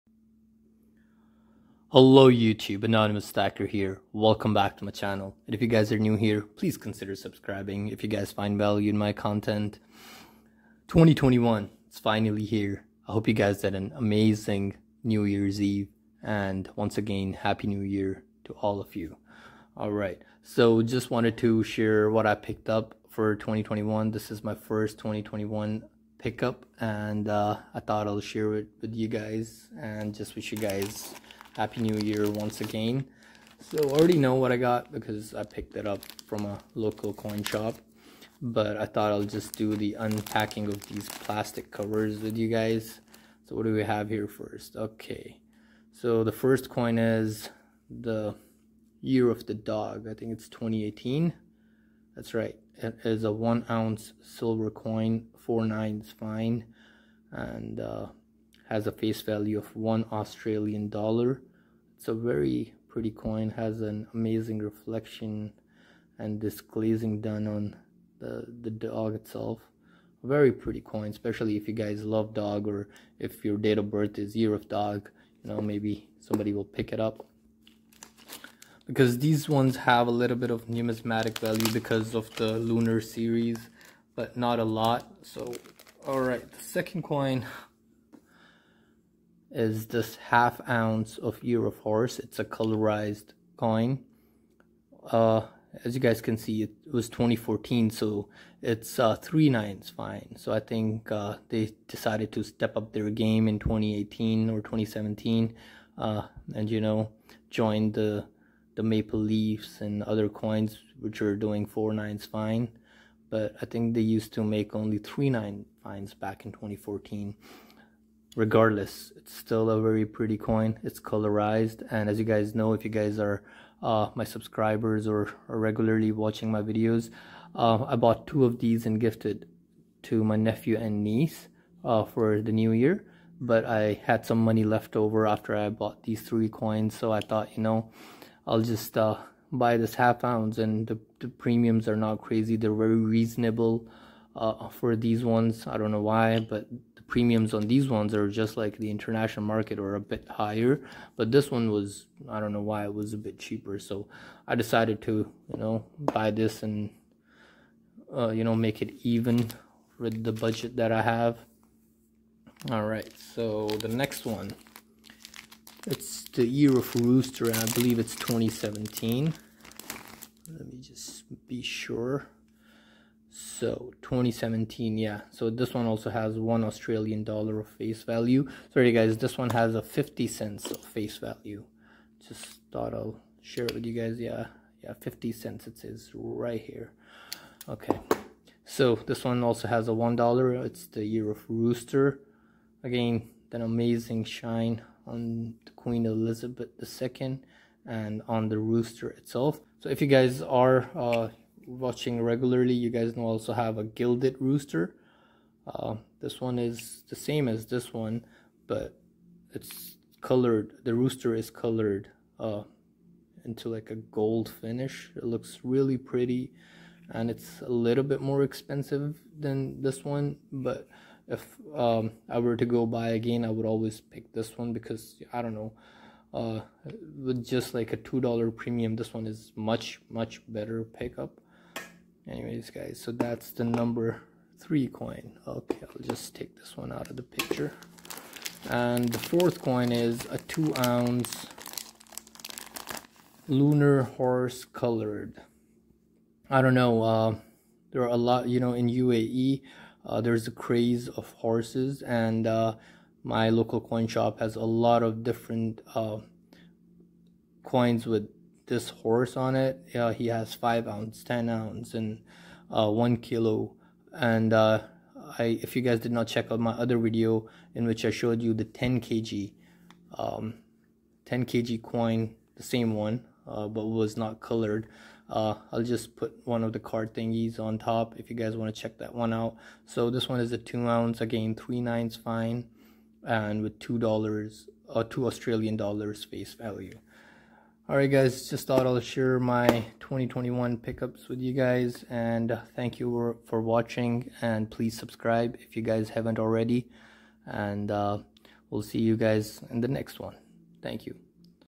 First 2021 Silver Coin Pick Ups Added to Stack! | Anonymous Stacker
Hello Everybody,

Hope you all had a wonderful New Year's Eve, I thought I'll share my first Silver Coins / Silver Bullion pick ups for 2021 with you all before I add it to my Silver Stack.

Anonymous Stacker